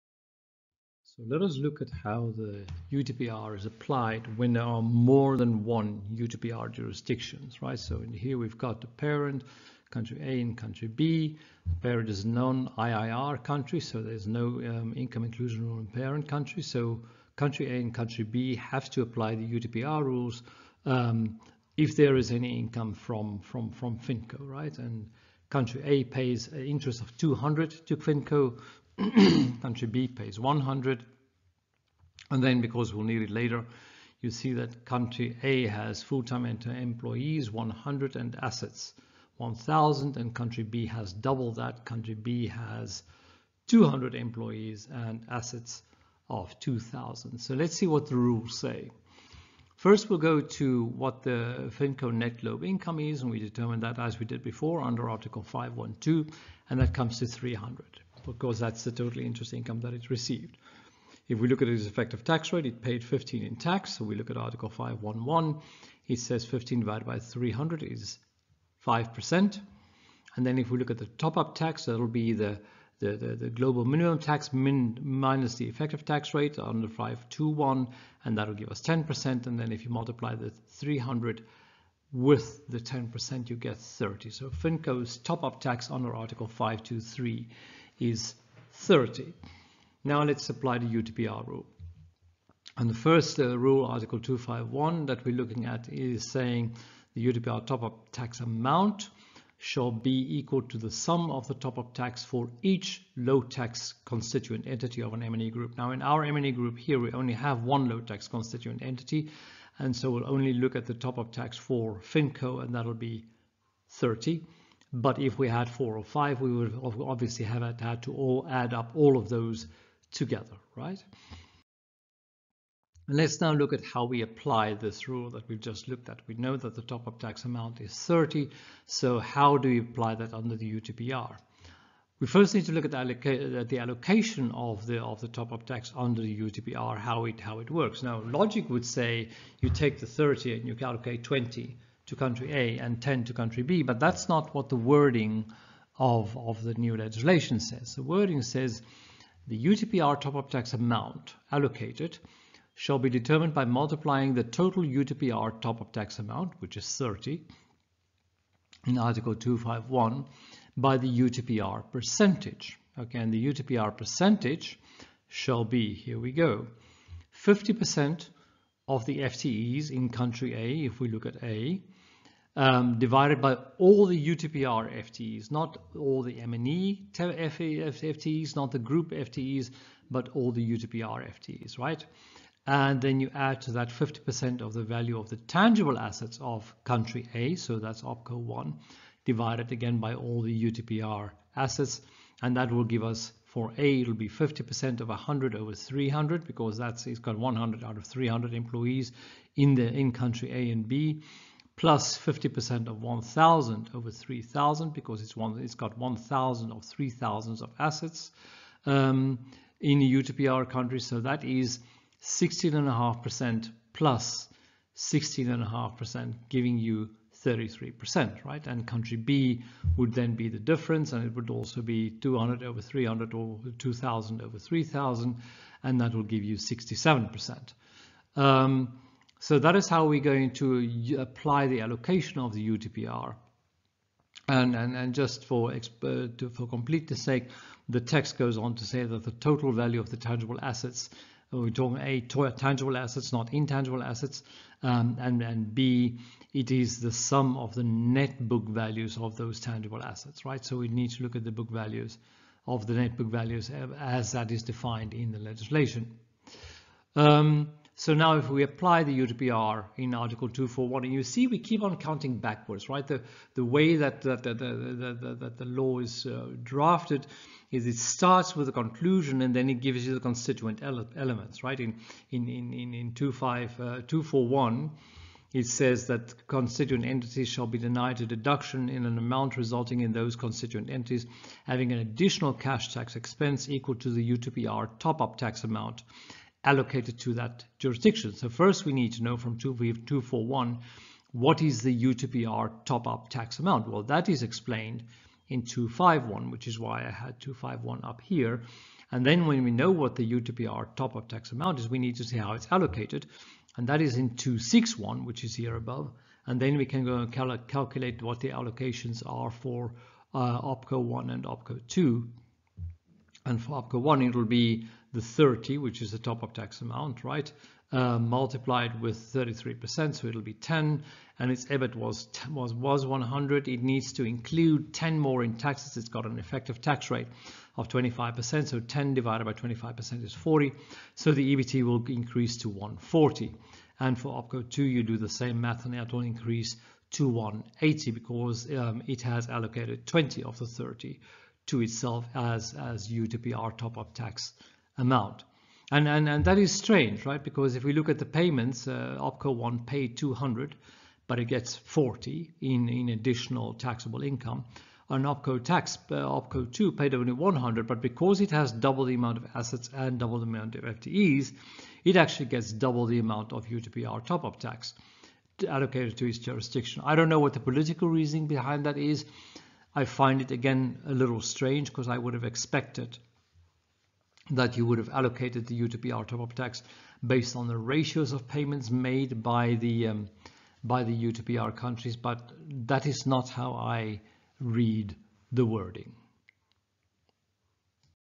Pillar 2. Allocating UTPR income
Question.  Am I reading the Pillar 2 Top-up Tax Amount to UTPR's correct?  This is how I interpret the December 2021 legislation.  It is not the way the October 2020 examples to chapter 7 of the 2020 Pillar 2 Blueprint worked.

It also does not seem to make sense to me:

In my simple example, one Low-Taxed Constituent Entity receives 200 from UTPR country A and 100 from UTPR country B.  However, if I follow article 2.6, then the UTPR Top-up Tax Amount allocated to Country A is half that of Country B, because country B has 2x the number of employees and the net tangible assets as country A.